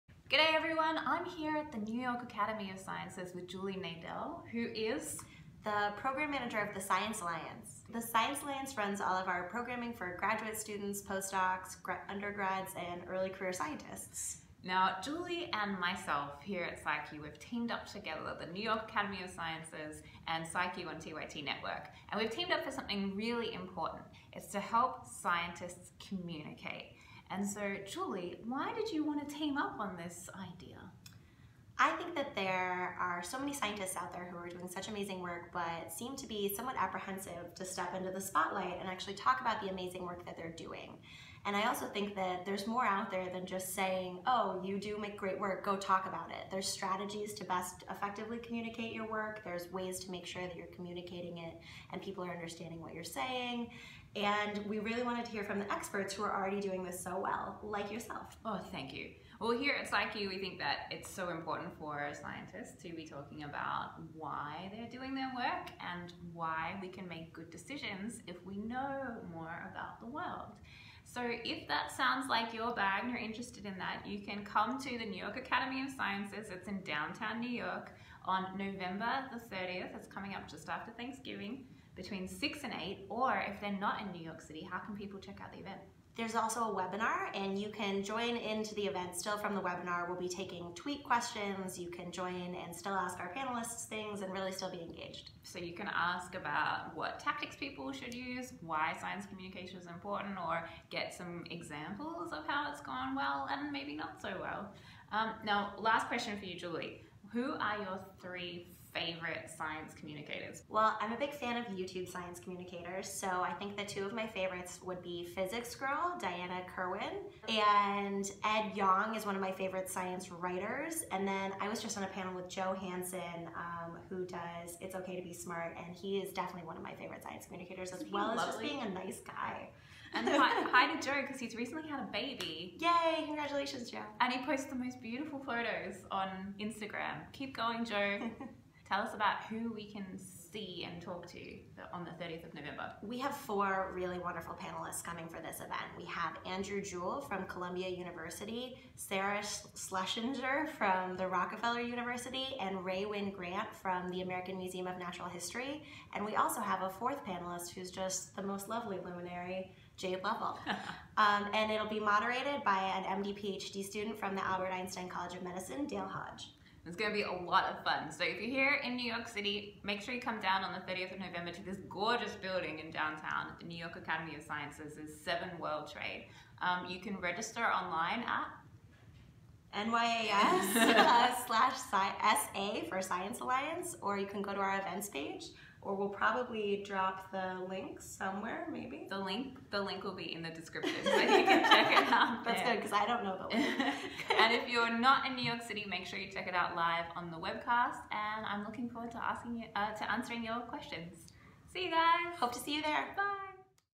Ninja science training this THURSDAY - join the team!
Every superhero needs training. So come along this Thursday for the ninja training course 'Communicating Science Effectively'!
It's run by New York Academy of Sciences and you'll learn some awesome communication tips - plus Charles Darwin will be there for a DarwinSelfie ;-)

If you're not able to make it to NY this Thursday, jump onto the webinar.

This video is presented by Jayde Lovell, at Youtube Space NY.